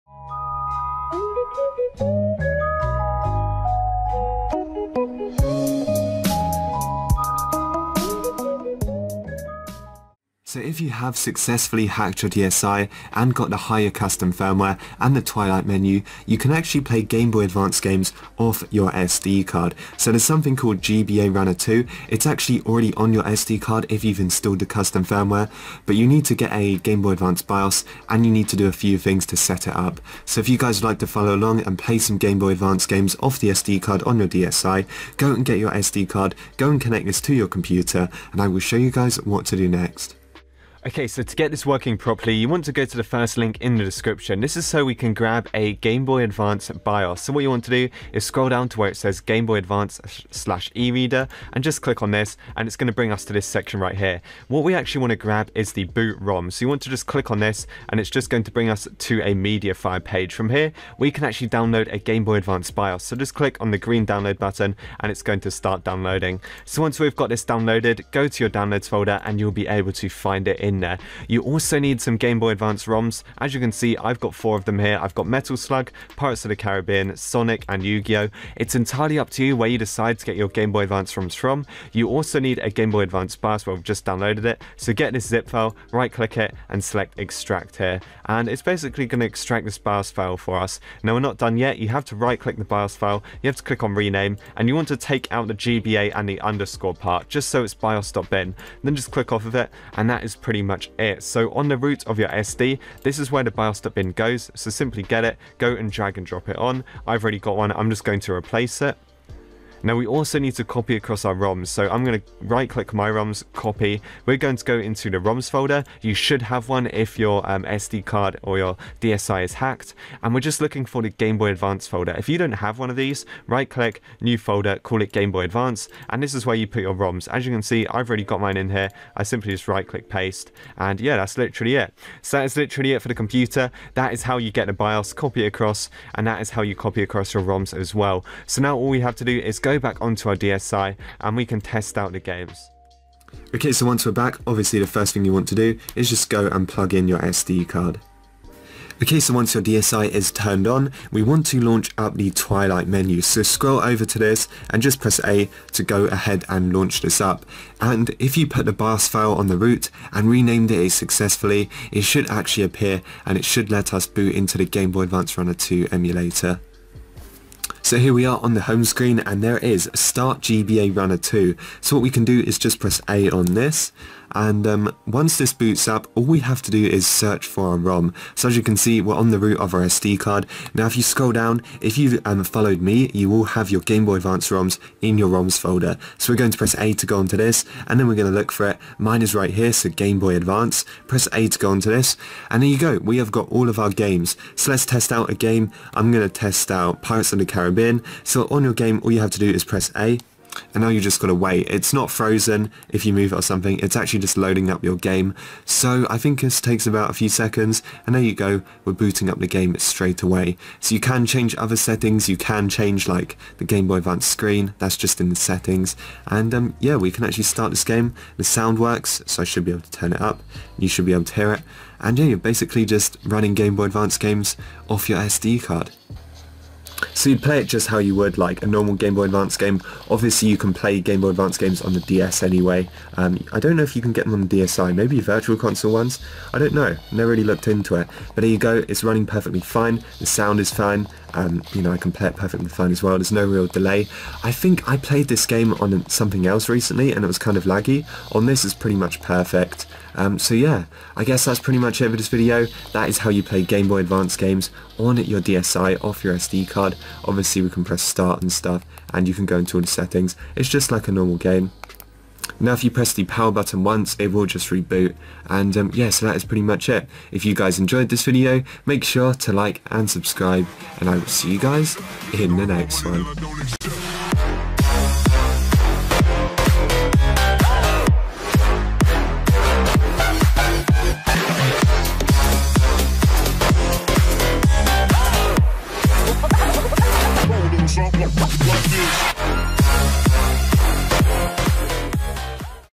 DSi GBA Games Off SD Card! GBARUNNER2!
How to play GBA games off your DSI’s SD card using the GBARunner2 software!